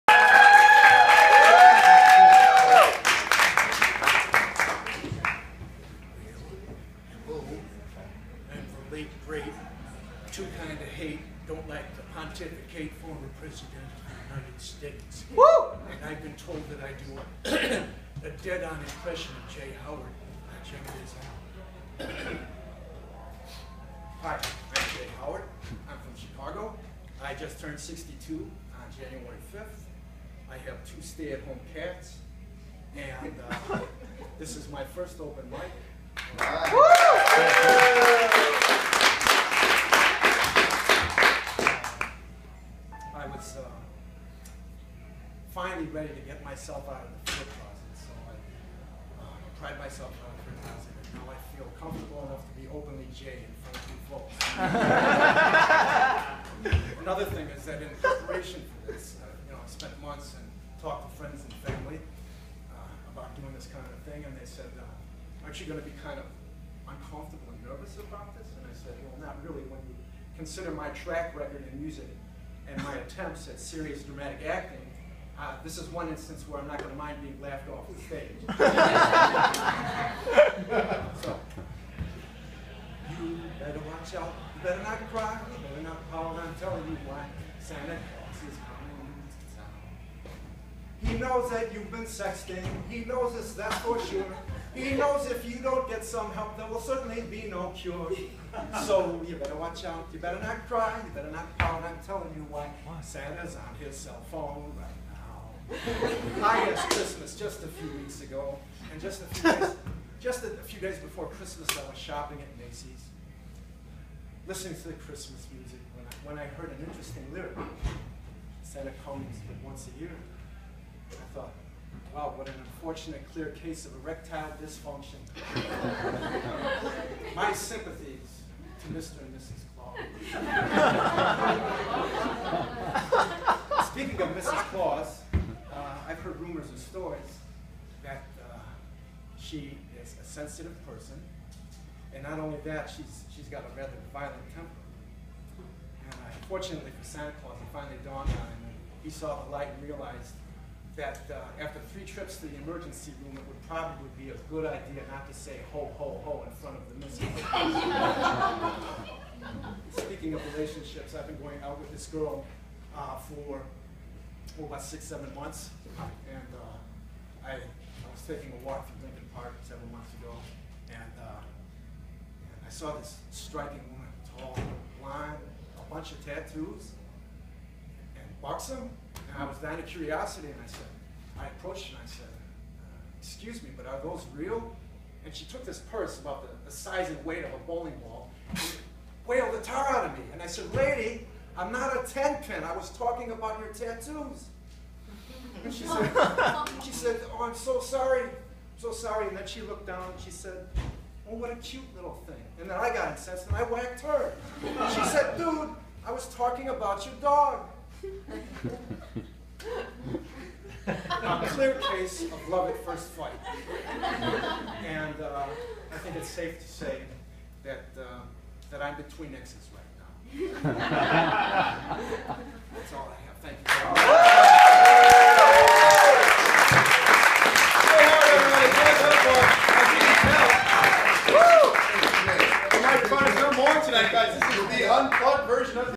Stand-up open mic debut at the Annoyance
Jan. 16, 2016 This video shows my very first open mic doing stand-up comedy.  It took place at the Annoyance Theater in Chicago.  The microphone was not working the night of this performance.  If you're having trouble hearing try using headphones or altering the settings on your stereo.

           Jay Howard (The "Next Jay!!?!??!!!")